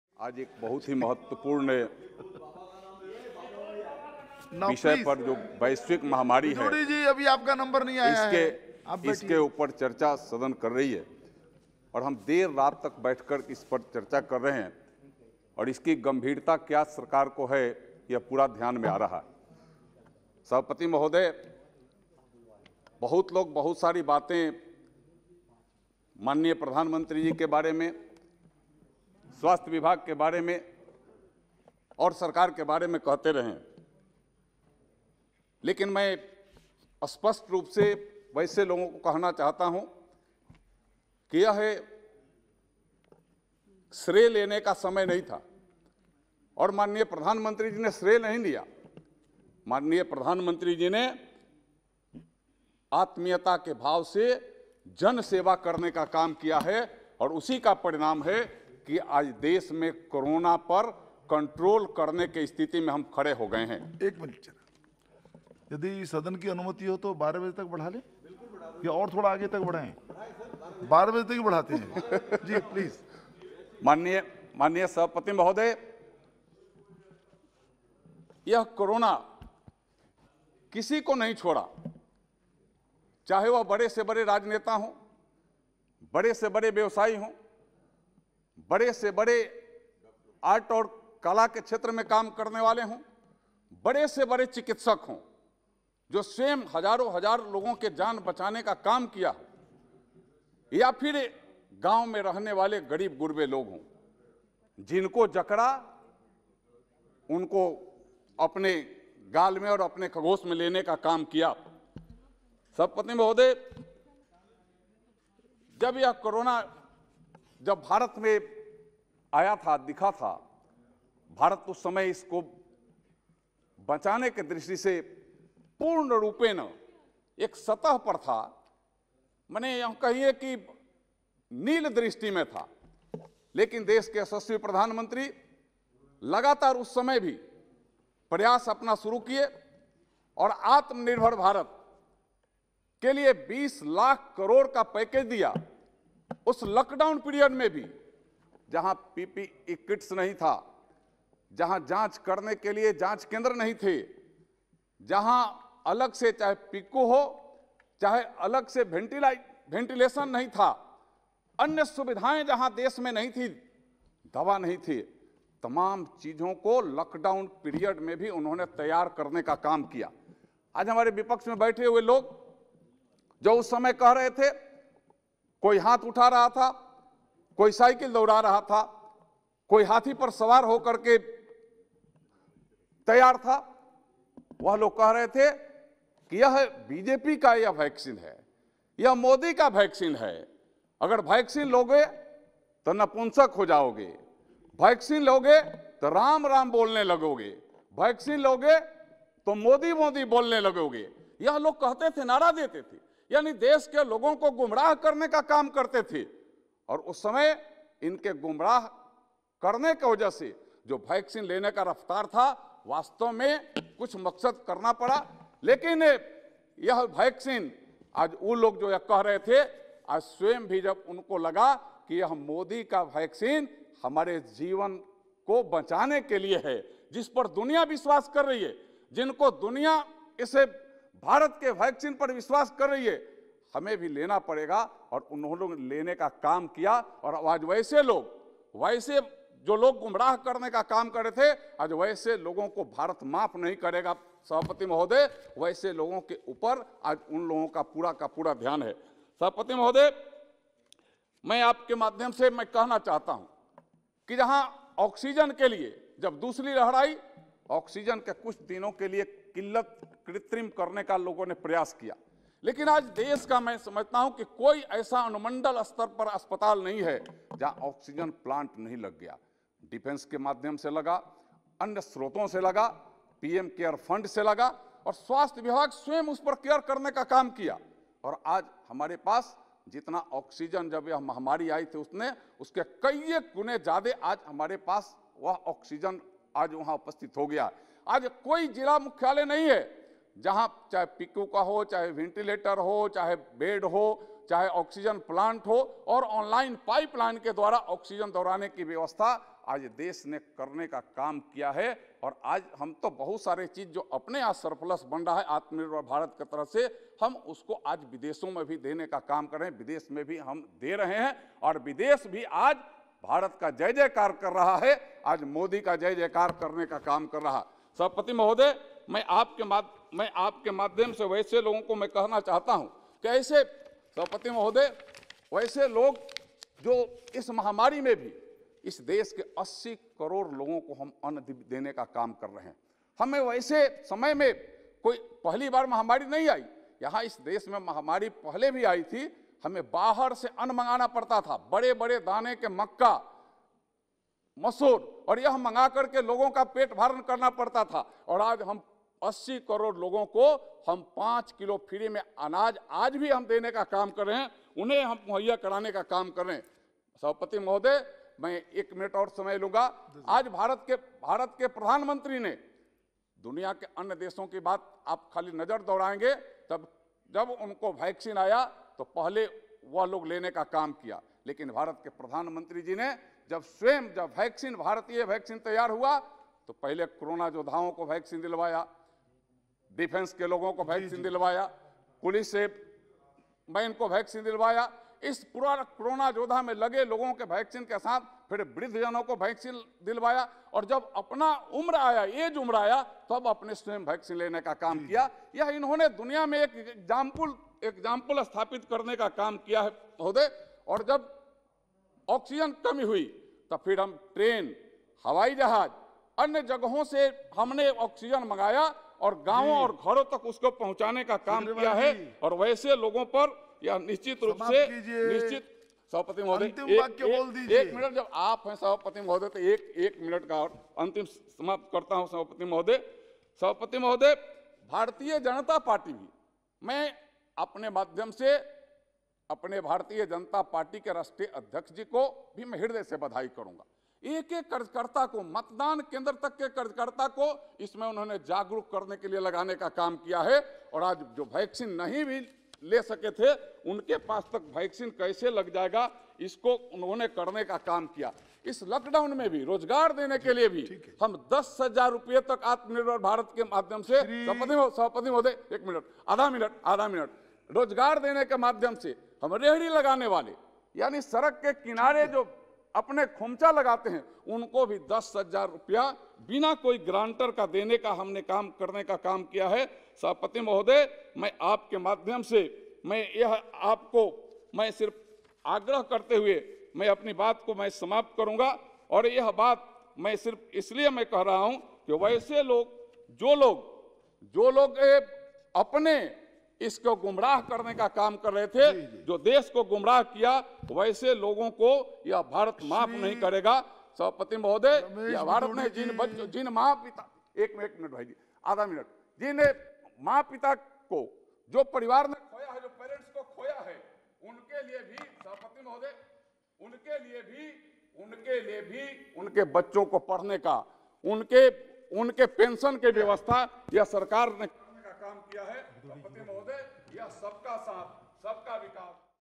Shri Janardan Singh Sigriwal on COVID 19 pandemic and various related aspects in Lok Sabha
Shri Janardan Singh Sigriwal on COVID 19 pandemic and various related aspects in Lok Sabha: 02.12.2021